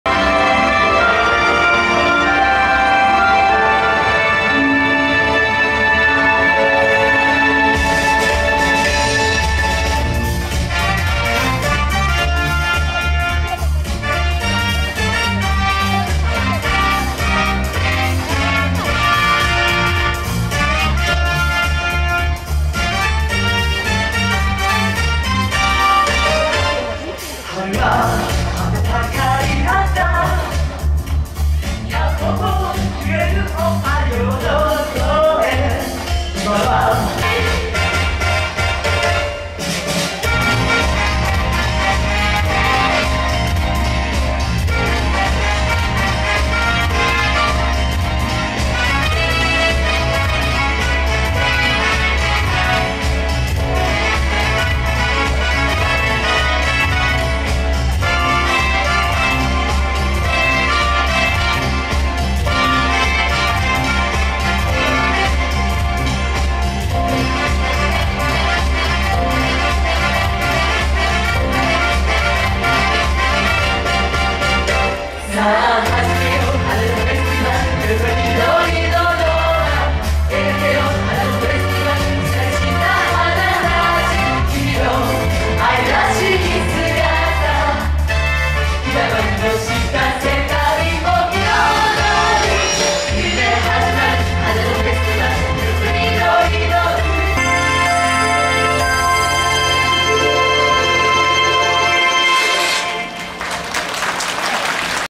20240402 4K チームウィング 蓮裕斗さん ハウステンボス歌劇団
ハウステンボス
歌劇(チーム翔)『Spring Festival Review Show ウィング ～色とりどりの春をあなたへ～』